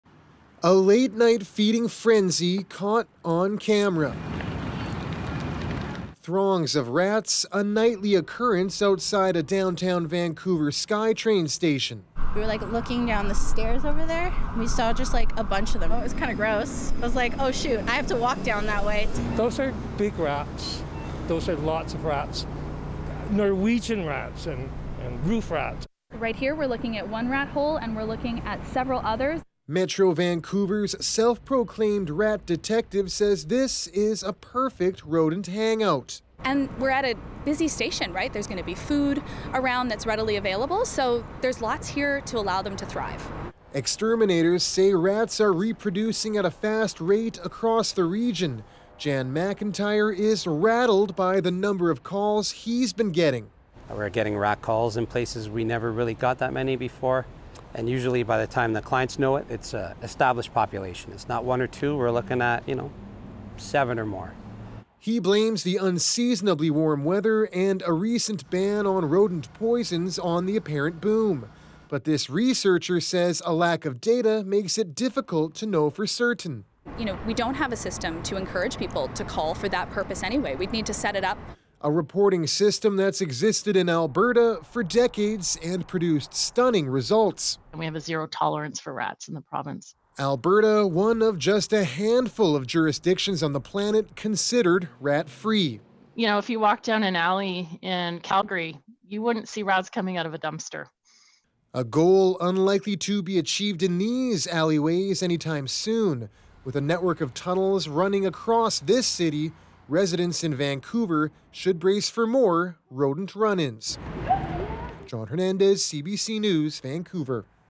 Vancouver’s rat population is booming
Exterminators say unseasonably warm weather and a ban on using rat poison are among the likely reasons that Vancouver is seeing an increasing number of rats.